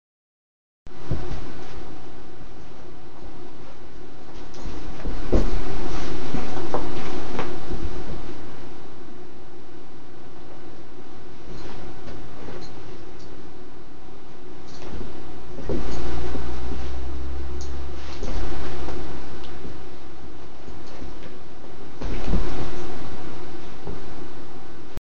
Doberman Pinscher Puppy Plays with Sheepskin Slipper
My Doberman Pinscher puppy McKenna loves to play with old slippers. Fortunately for me, she has turned out to be smart enough to differentiate between "her" slippers and "my" good shoes!